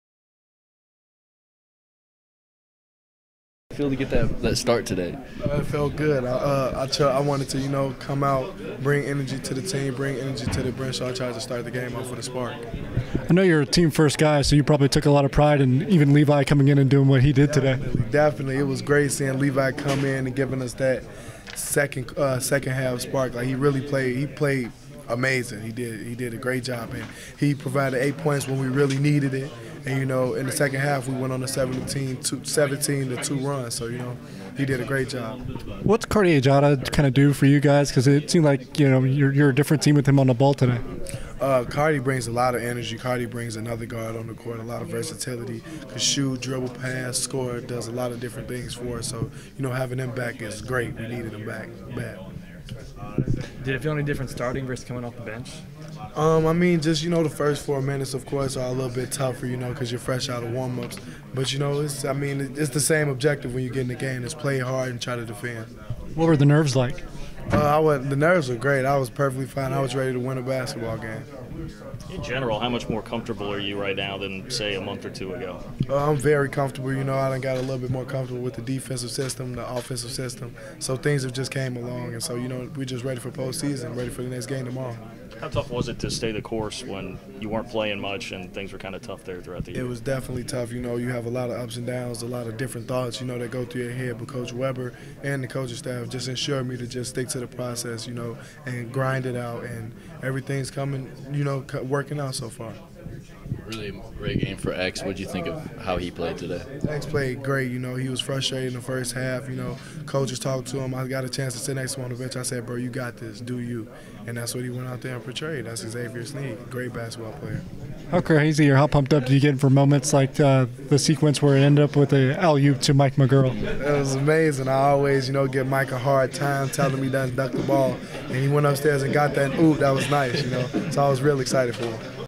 2019 Big 12 Tournament: Austin Trice After K-State vs TCU
Austin from the locker room after K-State beats TCU in Big 12 Tournament.